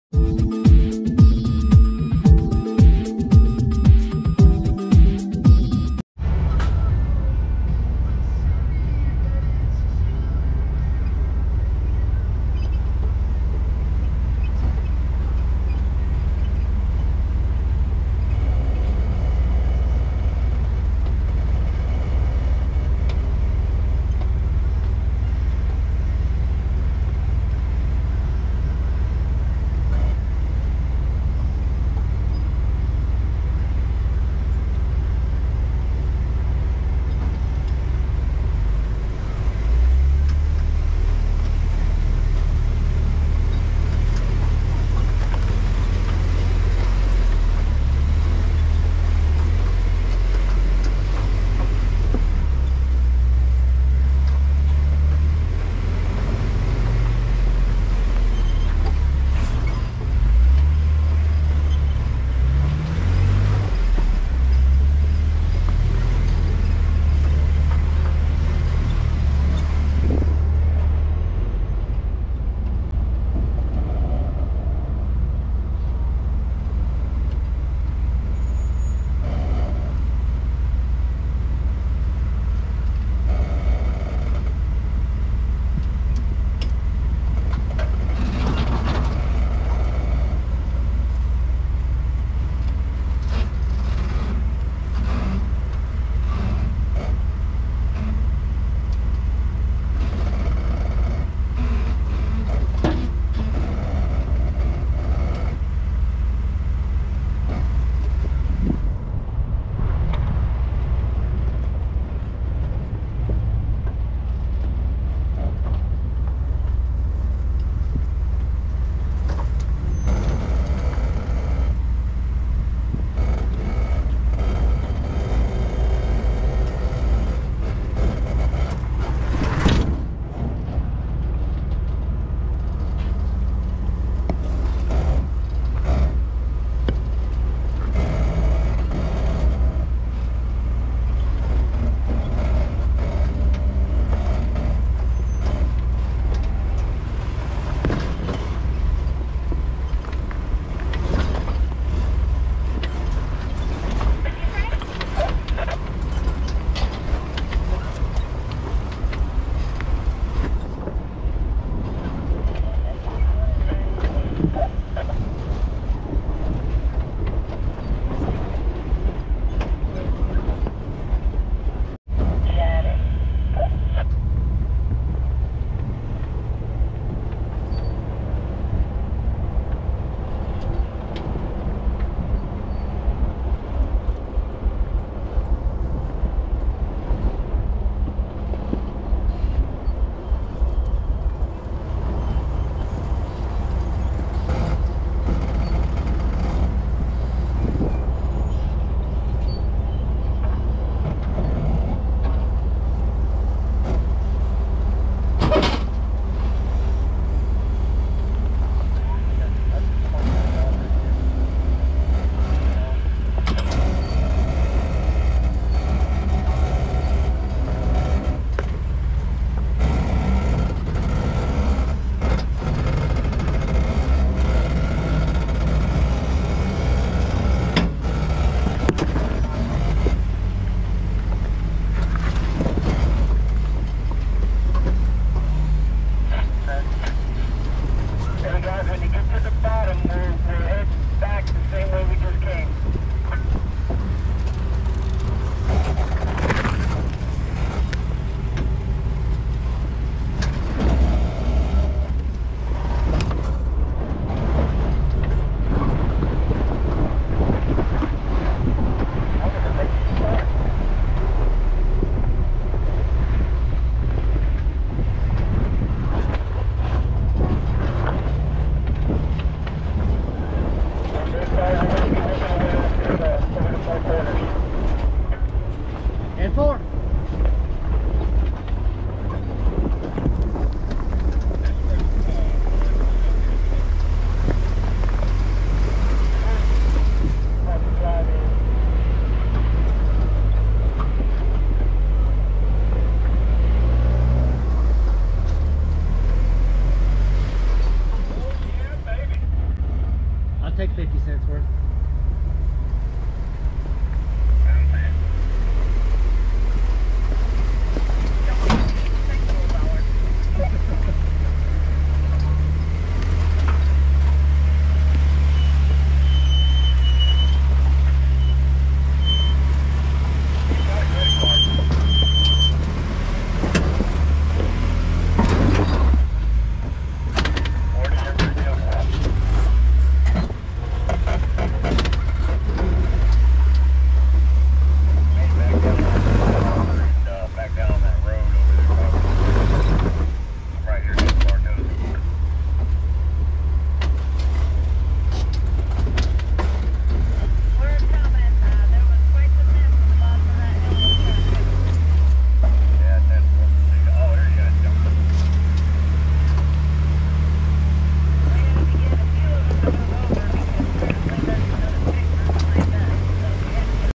Hidden Falls Adventure Park, November 17th-18th 2023 - Montage Part 2
Videos from various sections of trails all over the park.